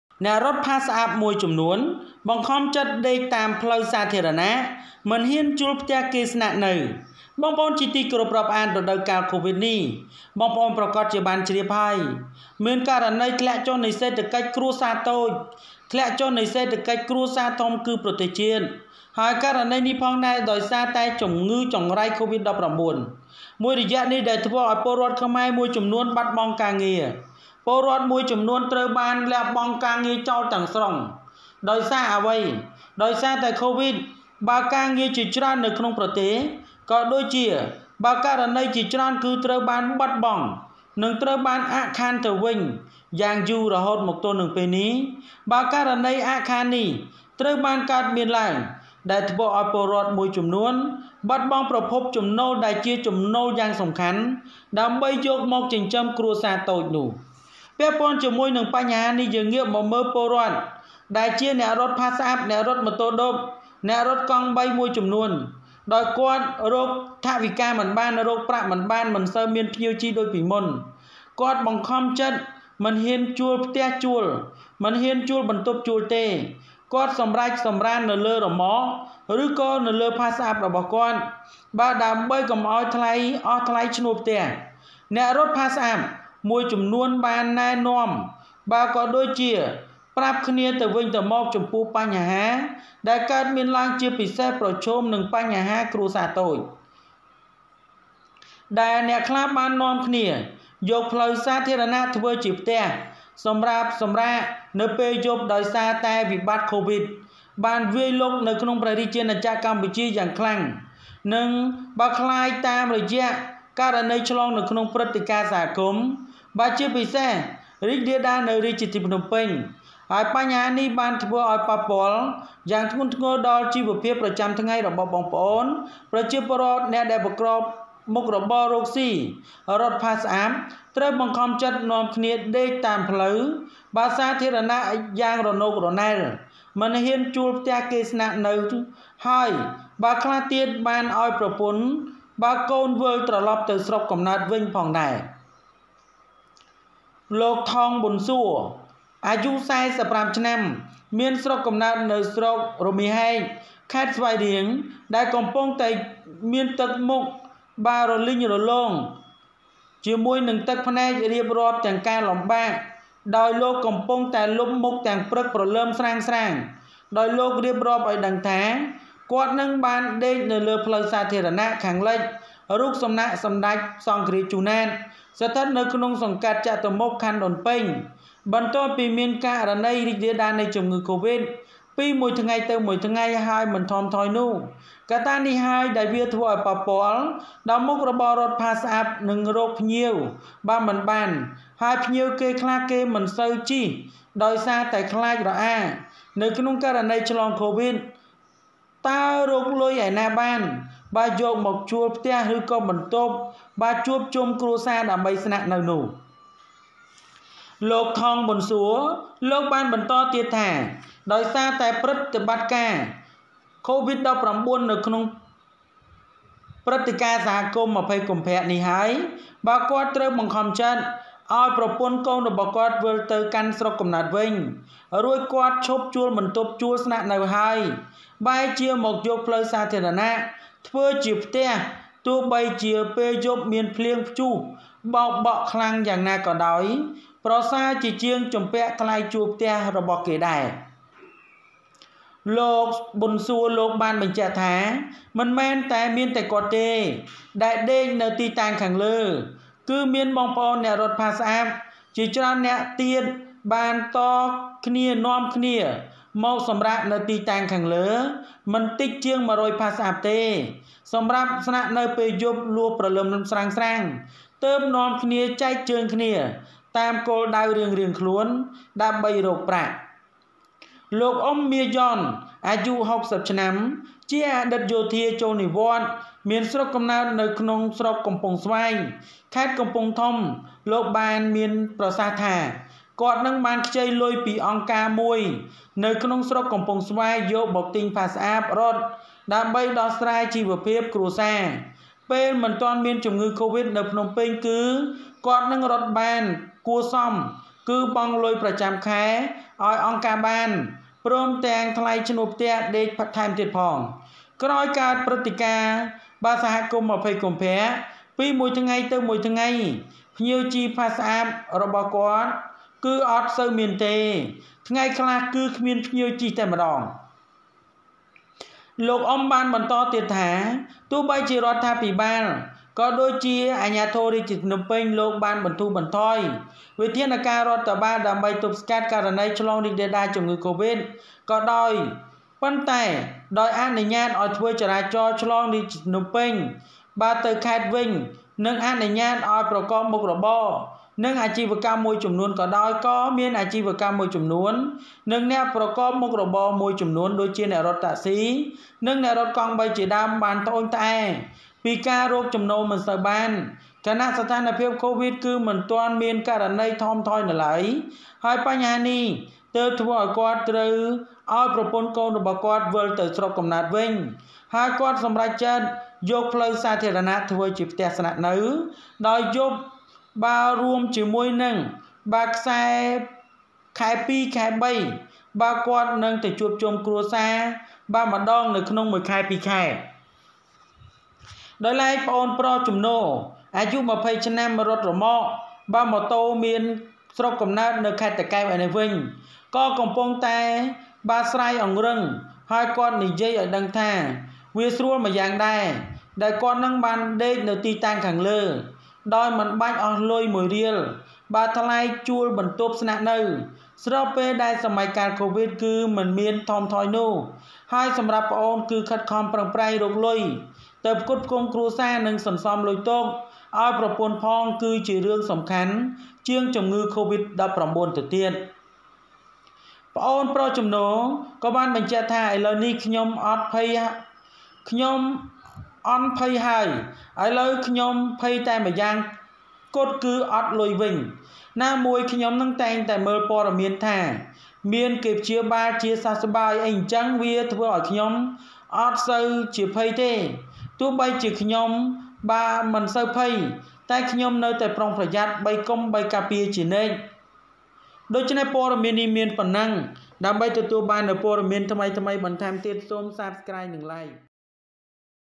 អ្នក​រត់​ផាស់​អាប់​មួយ​ចំនួន​បង្ខំចិត្ត​ដេក​តាម​ផ្លូវ​សាធារណៈ មិន​ហ៊ាន​ជួល​ផ្ទះ​គេ​ស្នាក់​នៅ
អ្នក​រត់​ផាស់​អាប់​មួយ​ចំនួន​បង្ខំចិត្ត​ដេក​តាម​ផ្លូវ​សាធារណៈ មិន​ហ៊ាន​ជួល​ផ្ទះ​គេ​ស្នាក់​នៅ Some runners forced to sleep on public roads, not daring to rent a house.